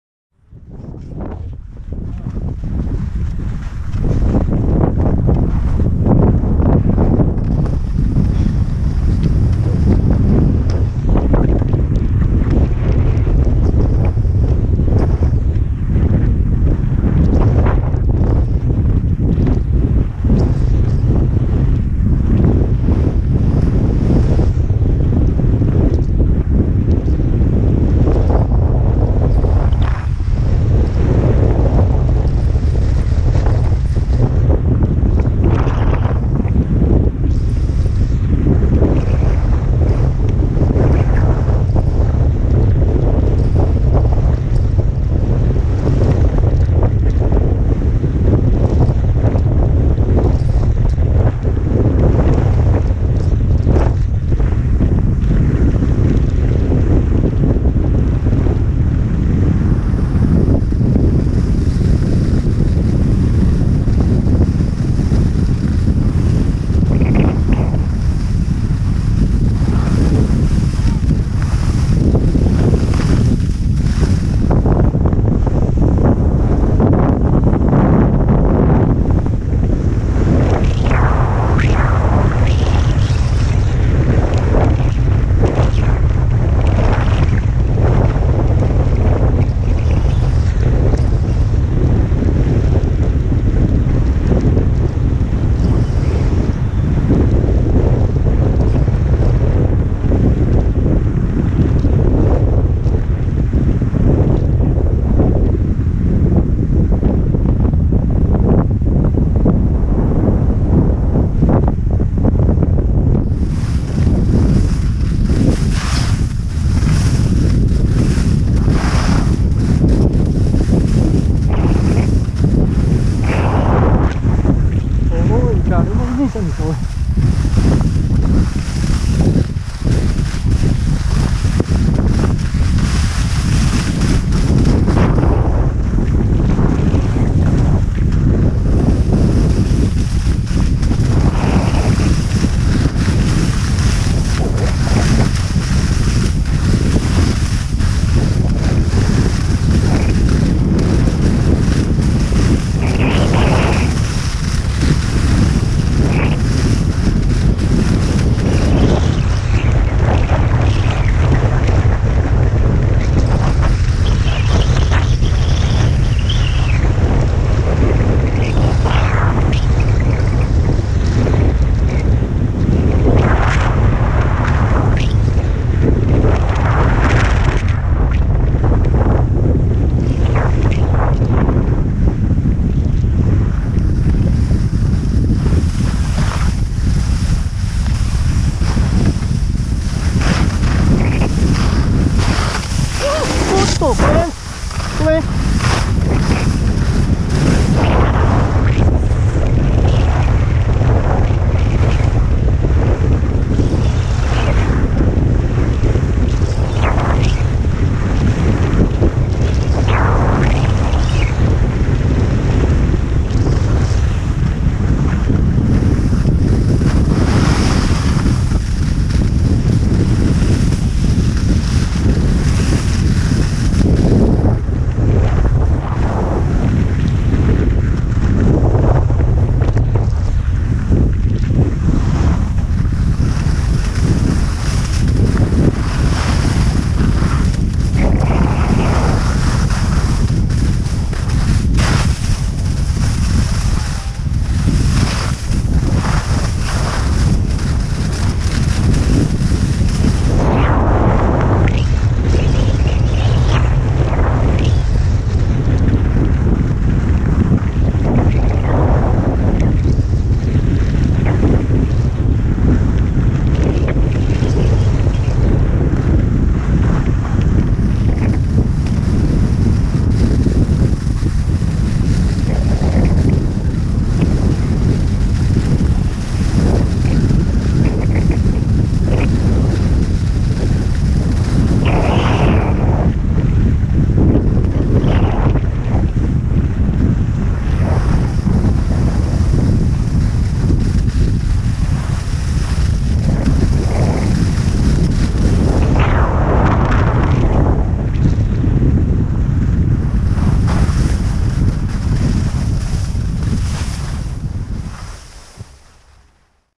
（パノラマ、サウスコース）志賀高原・焼額 20180325
右から来たブルーのウエアの女性、前を通り過ぎて驚かしちゃったかな。ごめんなさい。